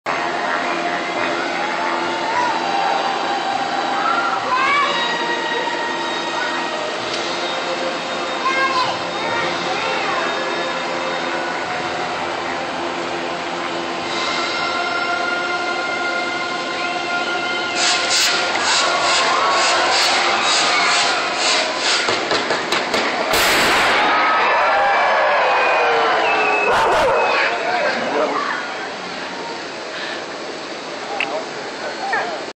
Olympic Cauldron Lighting Fail
Salt Lake City Relighting Ceremony. 10 years after the Olympics.